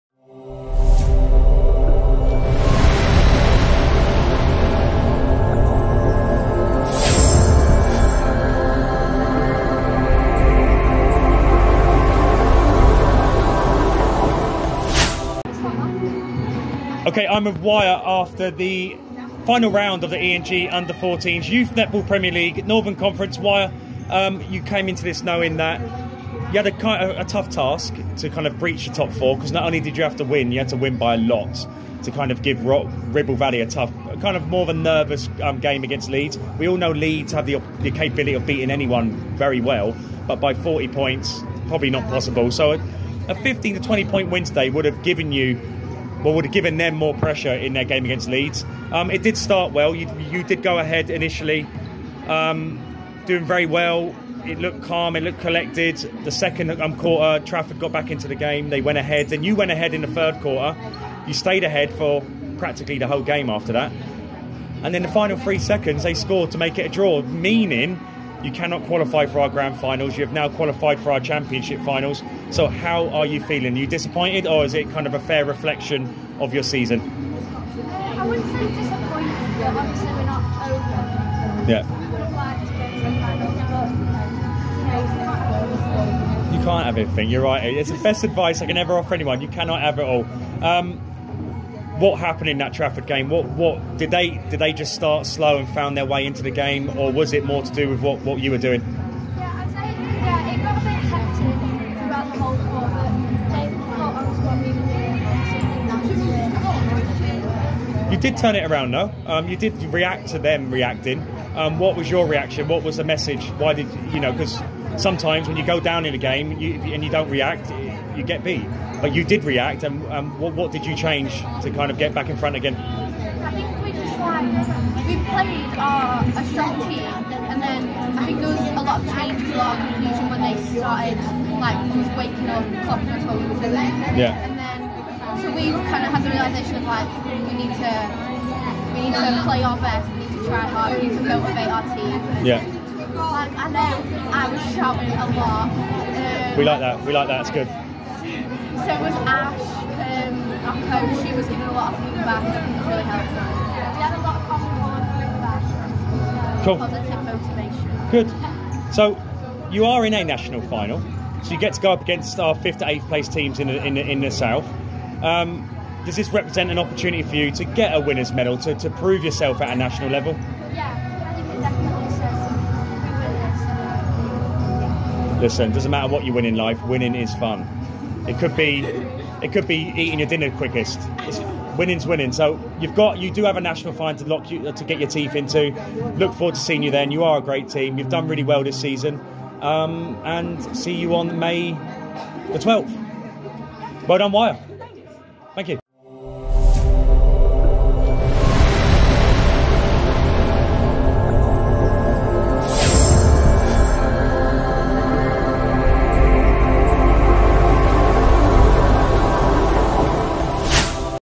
YOU CAN'T HAVE EVERYTHING
ENG U14 YNPL Northern Conference 2023/24
Wyre NC Vs Trafford NC
Round 5

Wyre NC missed out on winning this game after a last-second goal from Trafford meant they had to settle for a draw.